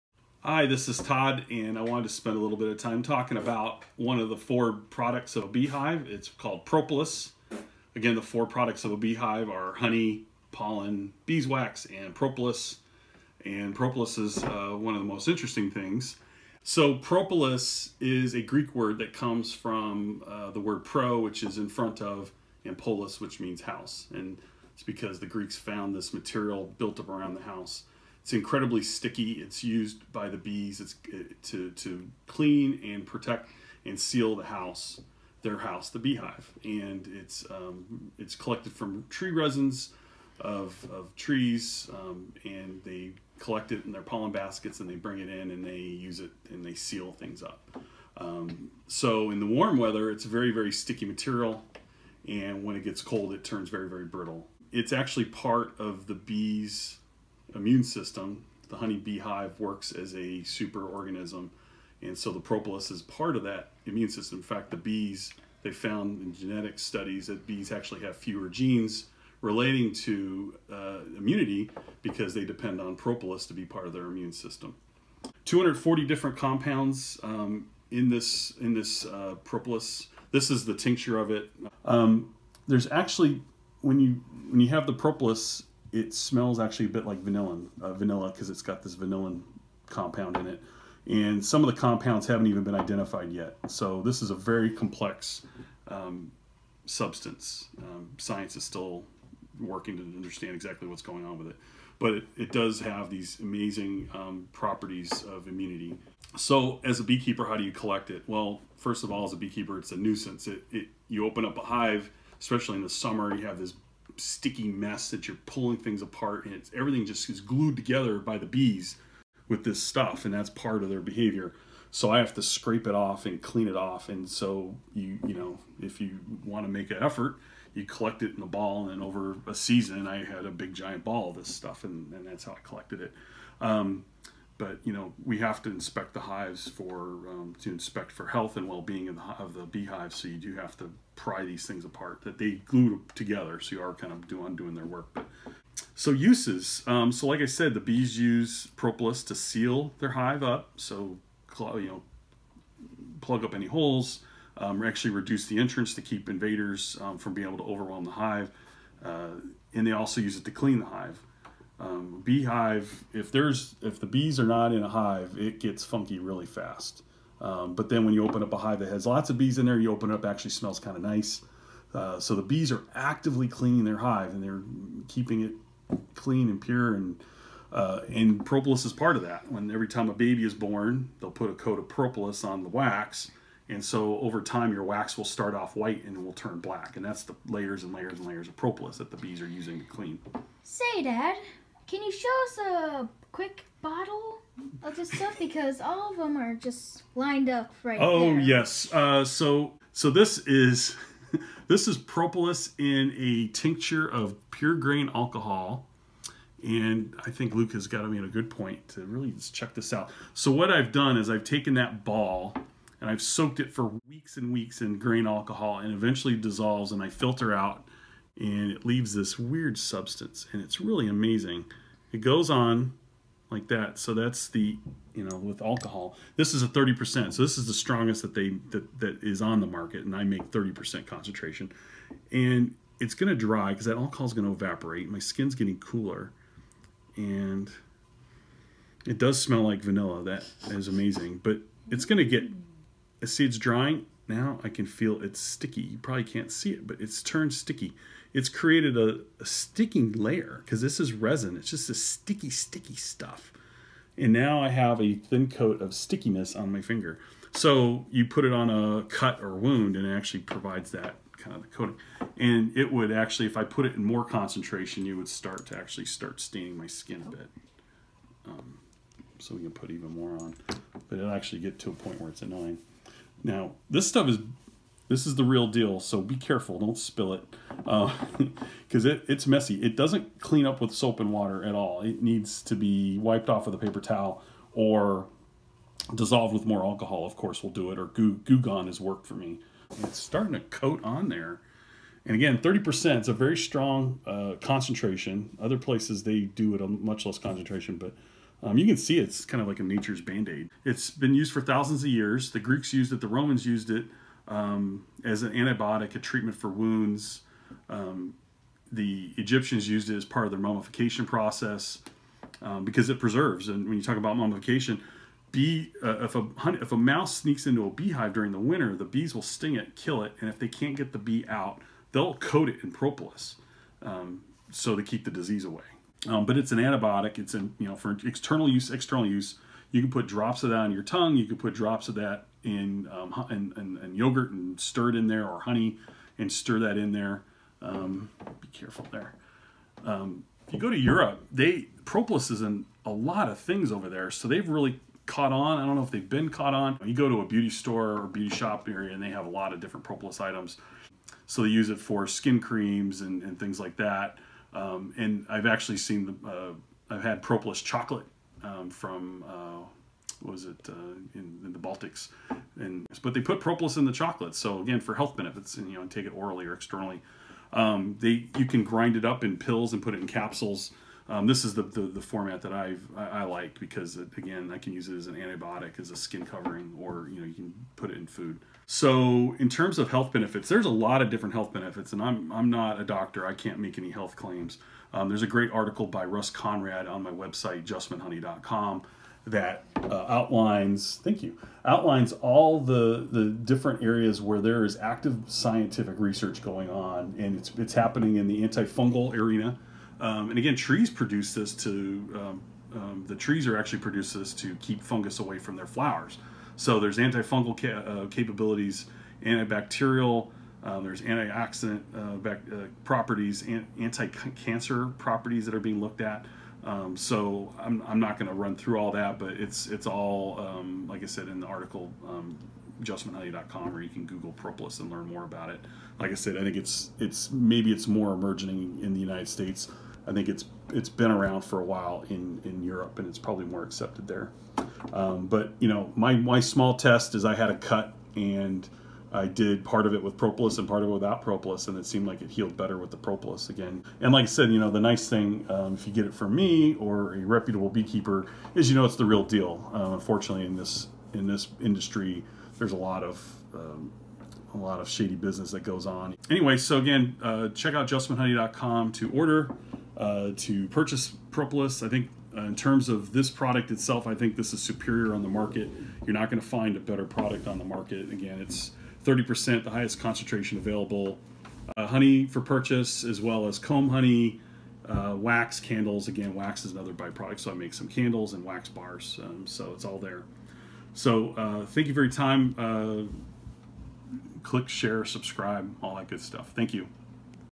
What is Propolis Tincture?
What is propolis?  Learn from a beekeeper who makes it.